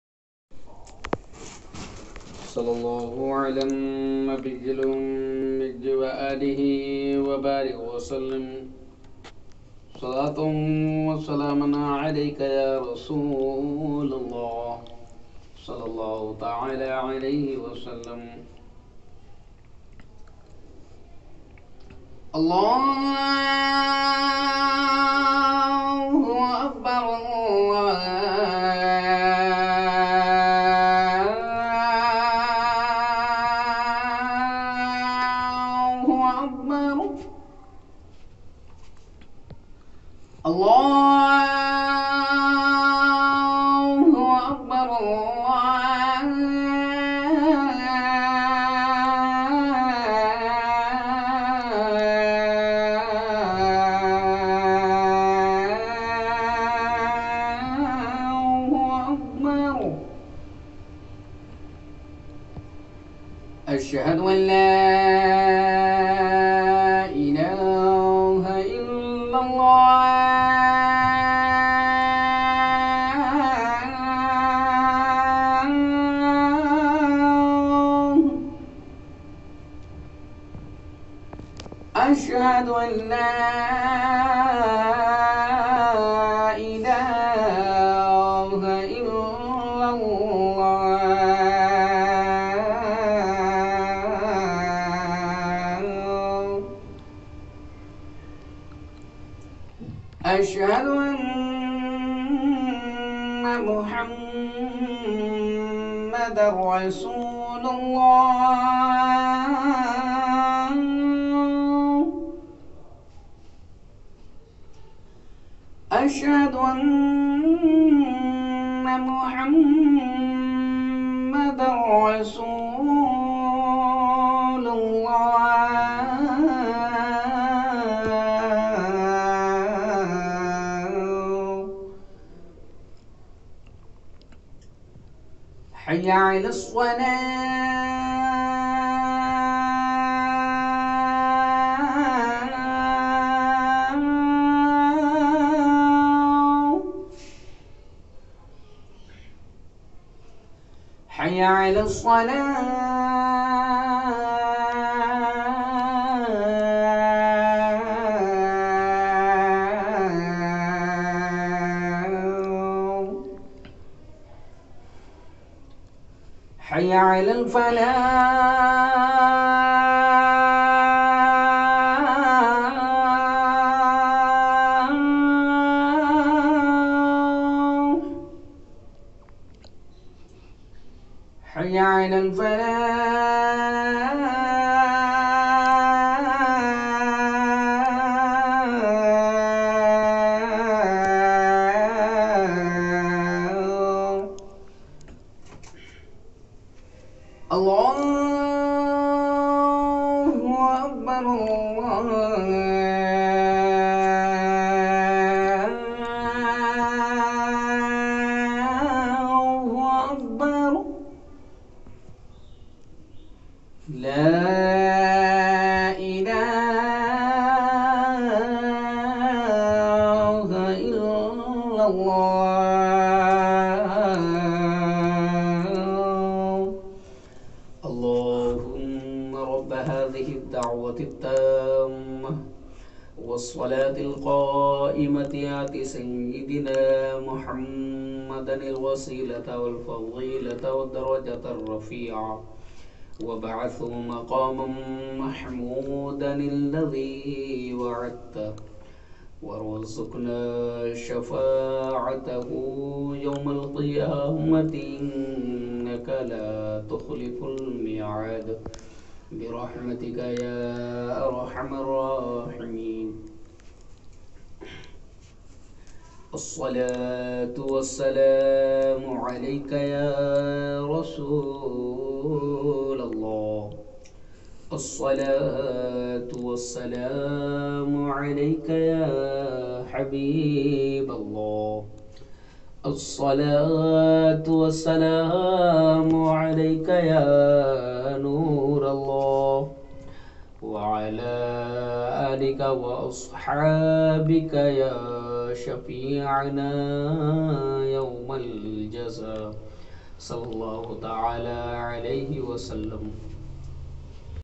Most Beautiful Azan |Sunni Juni Jama Masjid| (Recited By Muhammad Irshad Qari)
Copyright policy ;

I'm owner this video, and this is my Original content.

Owner of this mosque :- جماعت پروانی مصطفی